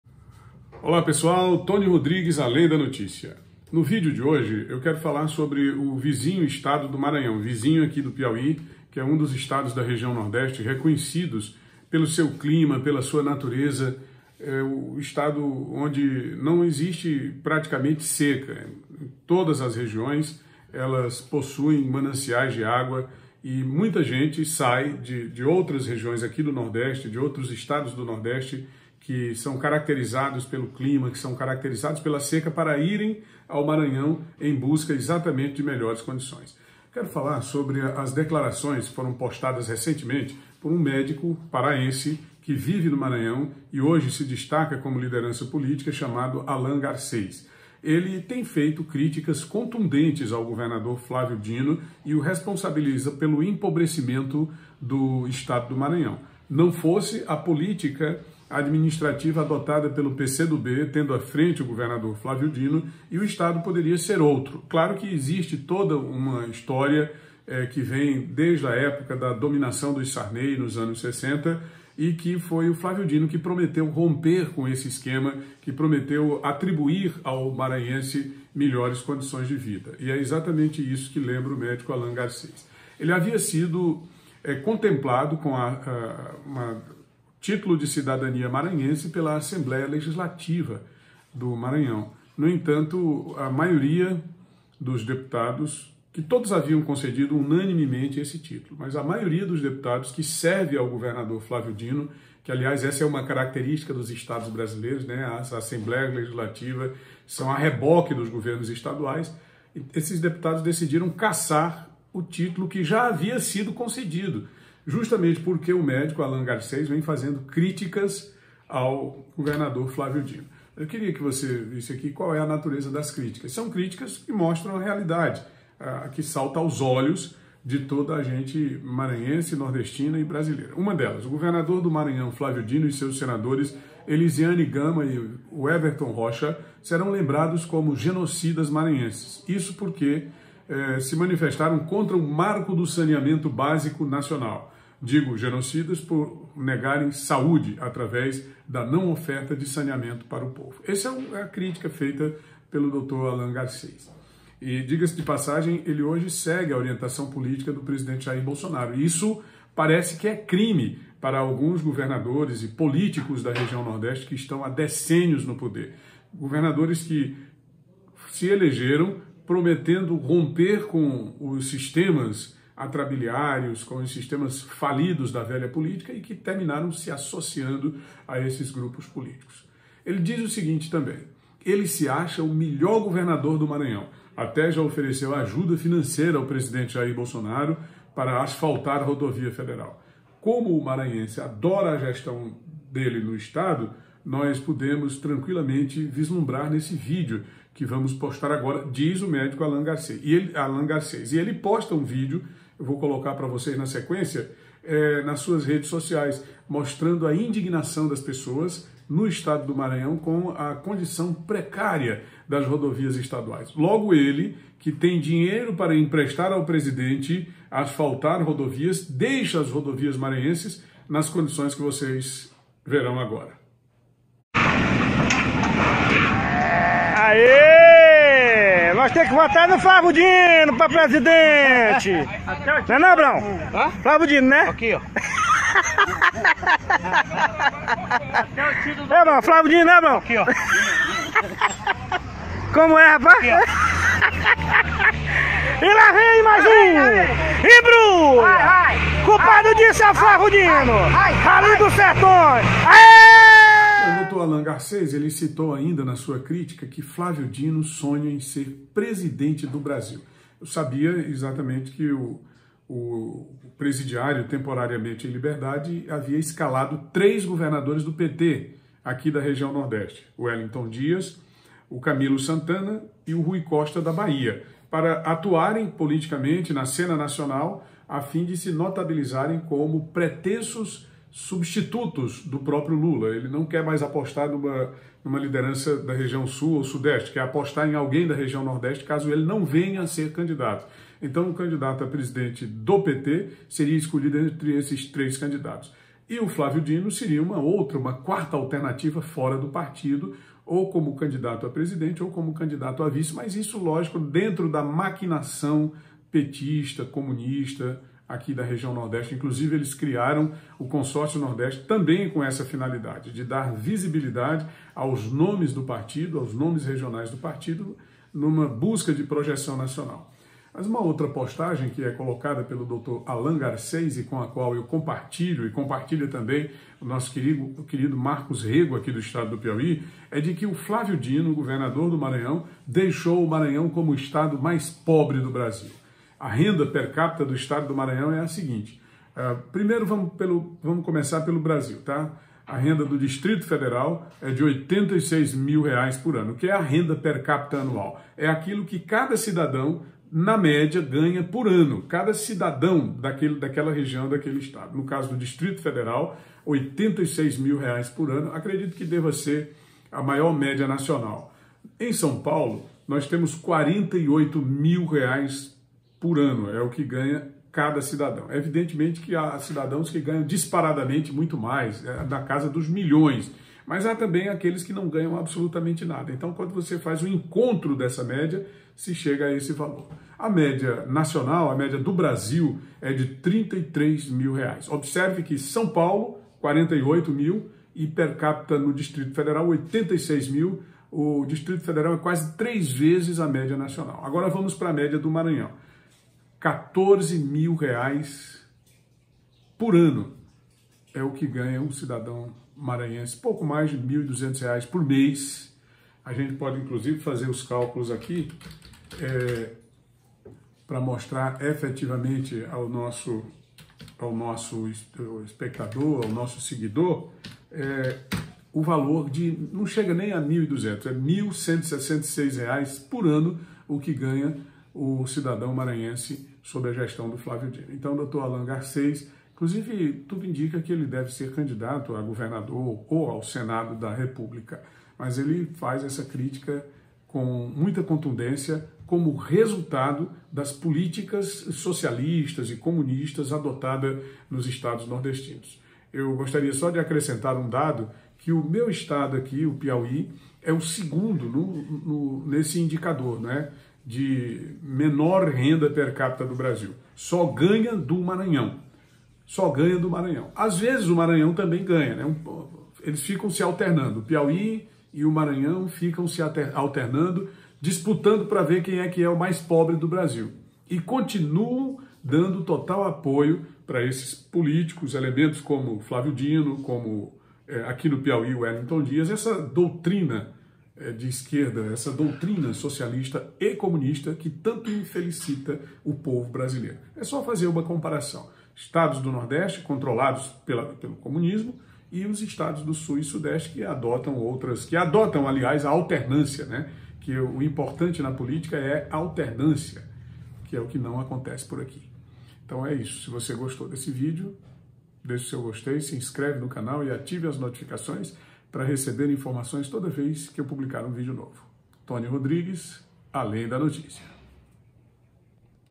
Flavio Dino torna Maranhão o estado mais pobre do Brasil
Em nosso vídeo de hoje: Flavio Dino torna Maranhão o estado mais pobre do Brasil, deixe os seus comentários.  Notícias, comentários e editoriais. Jornalismo e liberdade de expressão. Informações do Piauí, do Nordeste e do Brasil. Toni Rodrigues Além da Notícia. Obrigado.